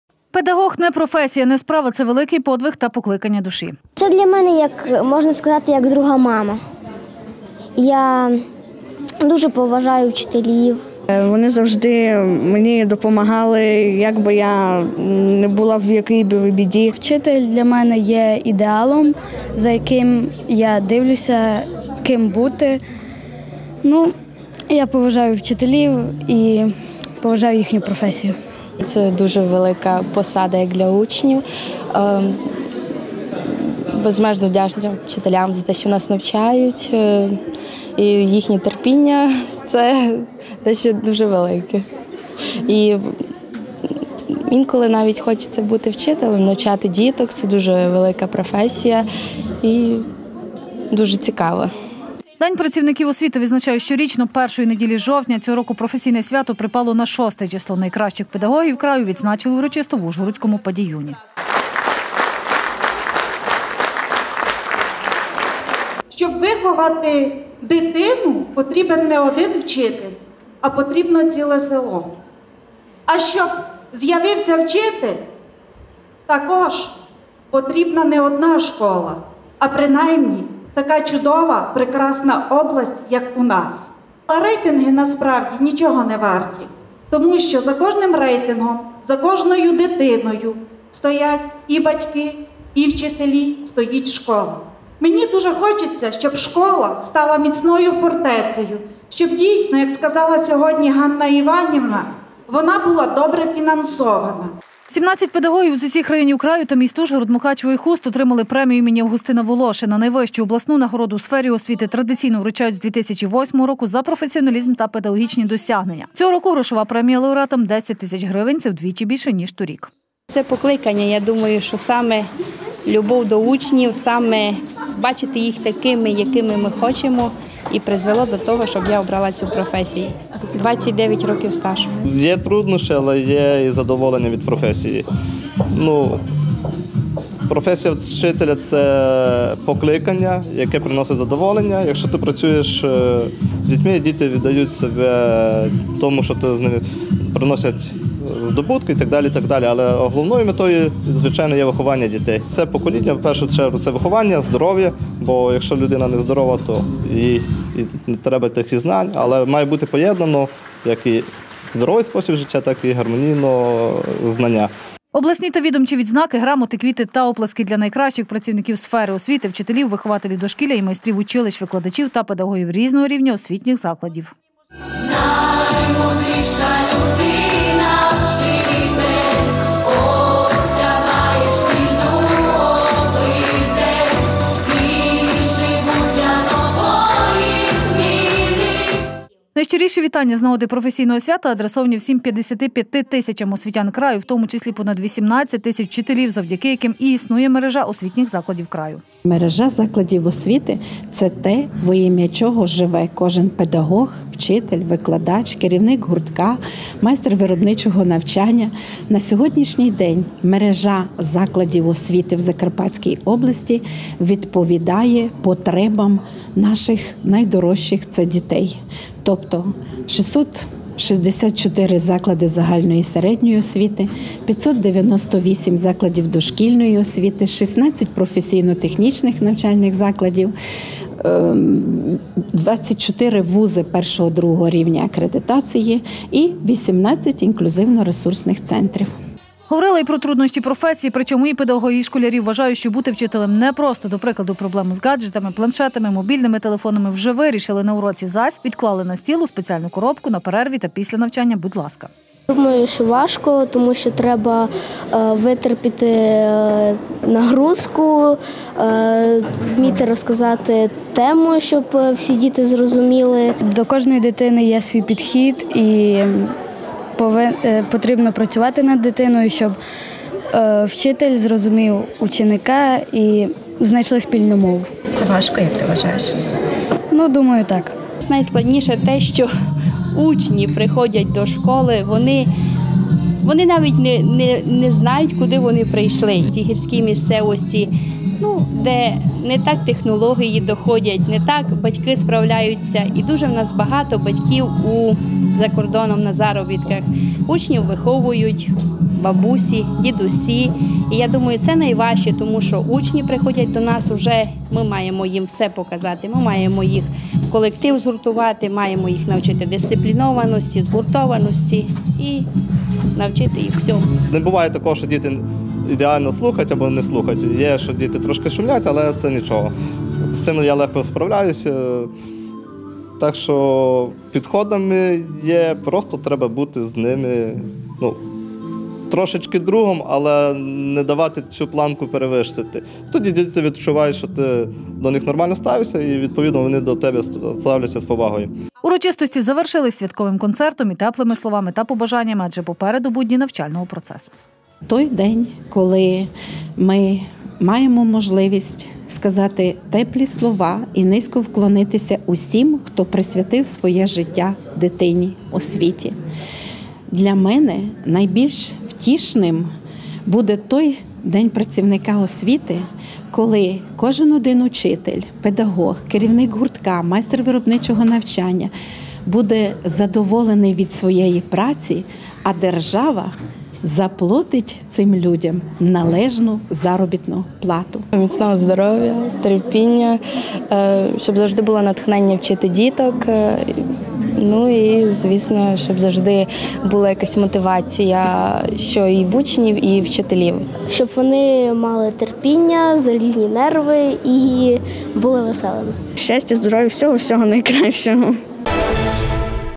День працівників освіти
День працівників освіти традиційно святкують першої неділі жовтня. Проте урочистості стартували вже в четвер. Так, кращих працівників освітньої галузі Закарпаття нагородили в Ужгороді. З нагоди професійного свята у ПАДІЮНі педагогам краю вручили обласні та відомчі відзнаки, а також щорічну премію імені Августина Волошина. Це найвища обласна нагорода у сфері освіти підкріплена грошовою премією. Цього року розмір її подвоїли – по 10 тисяч гривень отримали 17 освітян від кожного району.